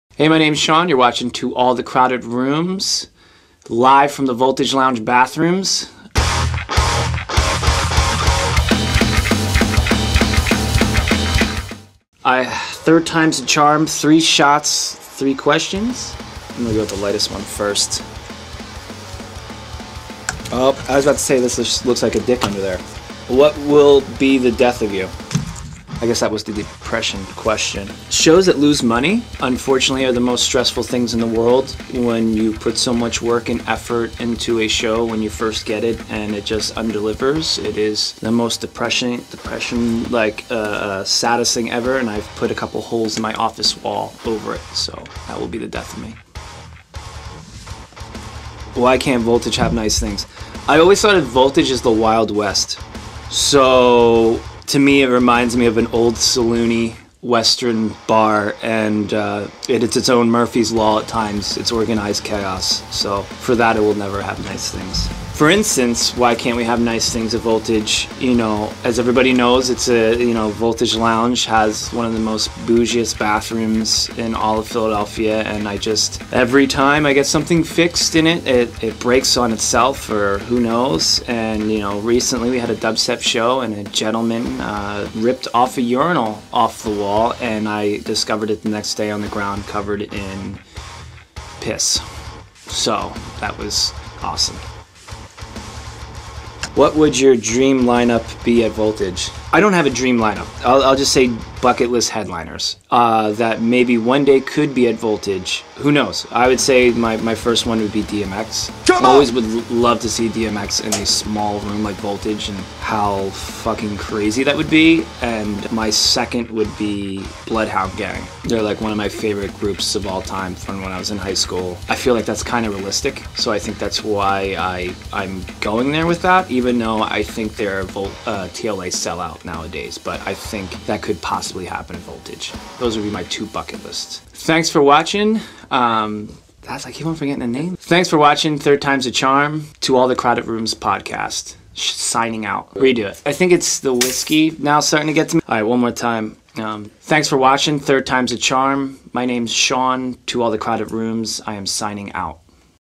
Taking Shots With Sean Salm From Voltage Lounge and Sad & Boujee
We are debuting our NEW segment called "Third Times The Charm," where the guest takes three shots, then answers three questions. We had Sean Salm creator of @sadandboujeeparty and talent buyer at the Voltage Lounge come on and answer some questions. Stay on the look out for the full podcast episode coming out this week!

Filmed/Edited/Graphic Design: @jmetzdesign
Audio: Mikey Lince